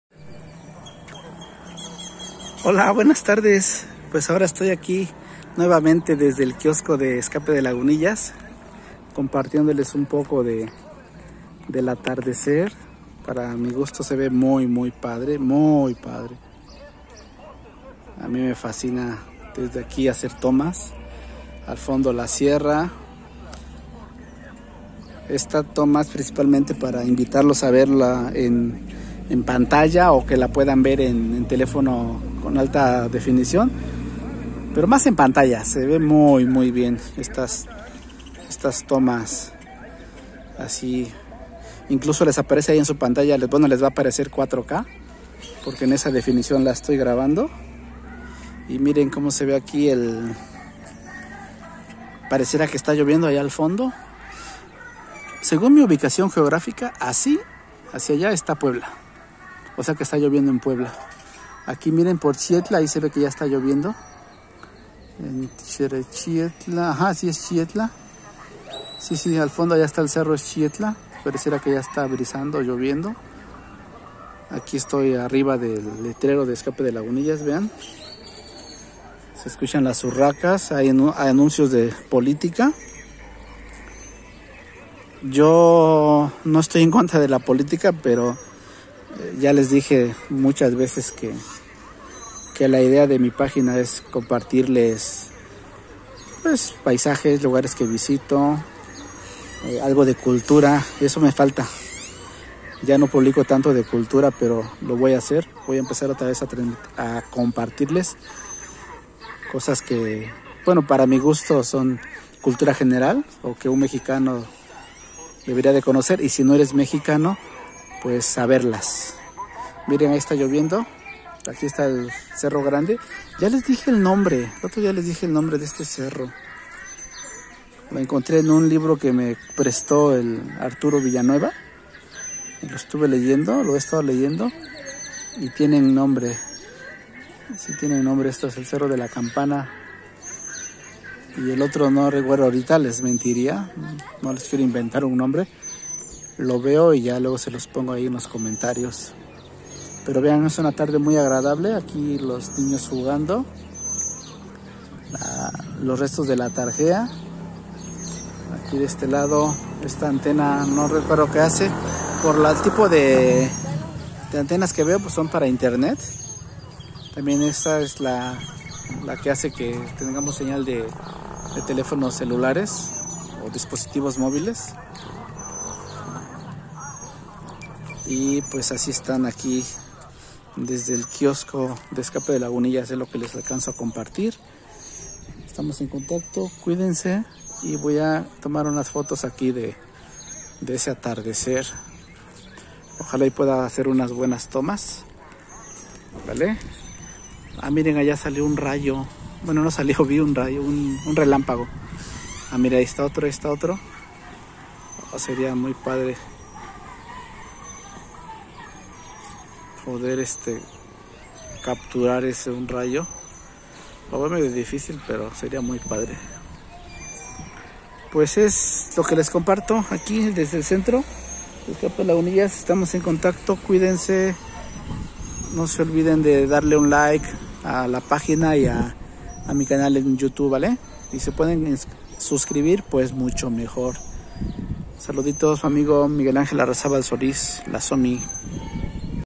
Zócalo Escape de Lagunillas
Vista preciosa desde el kiosko de Escape De Lagunillas Chietla Puebla México para el mundo. 28 de mayo de 2021. LASOMI